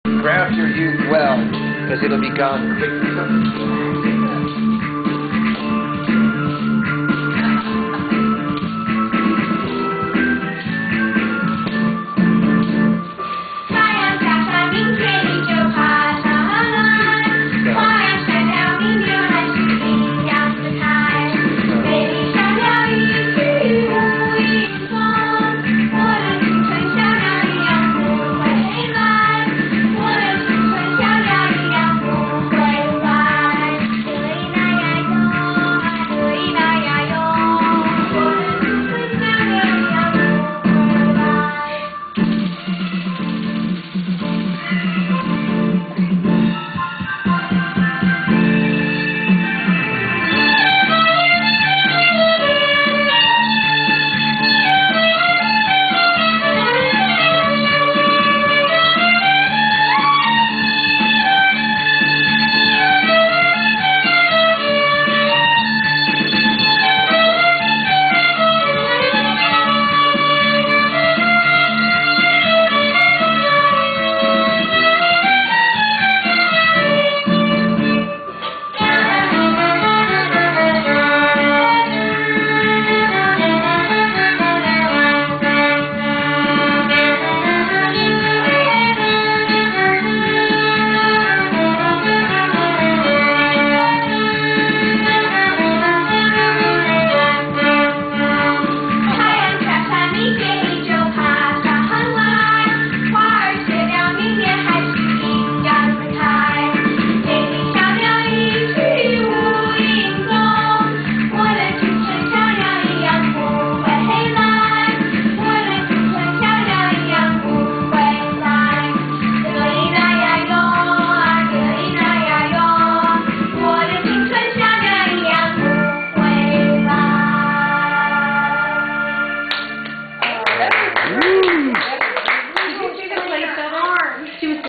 SDA13 - Swim & Water Polo
Swim and Water Polo at Summer Dreamers Academy. Game day on week 1.